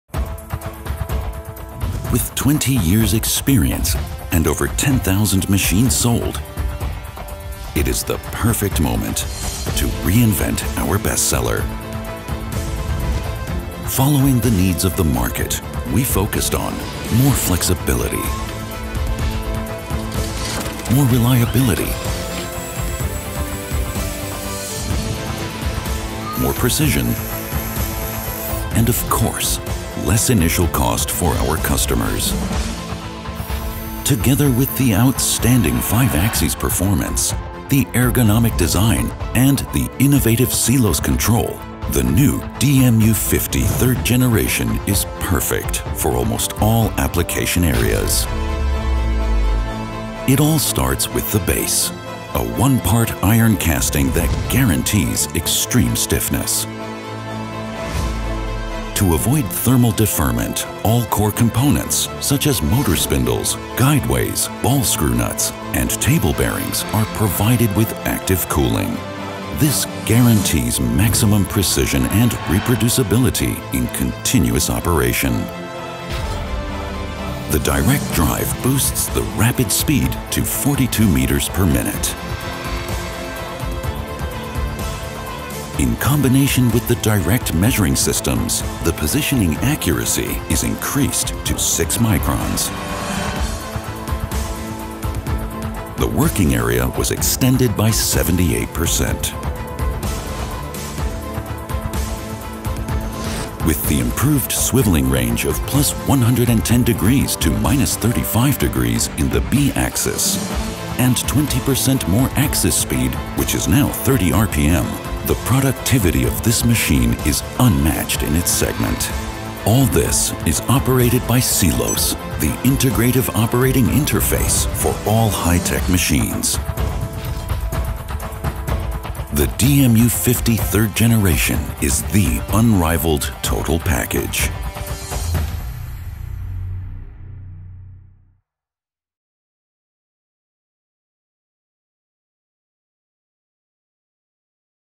DMU 50 3rd. Generation - 5-axis machining with speedMASTER® as standard
The new DMU 50 3rd Generation from DMG MORI combines more than 20 years of experience in 5-axis milling with unique future technologies and sets new standards in simultaneous 5-axis machining. With its enlarged swivel range, more powerful spindle and its innovative cooling concept, the world premiere ensures competitive ability in many areas of application for demanding branches such as the aerospace, medical and automotive industries.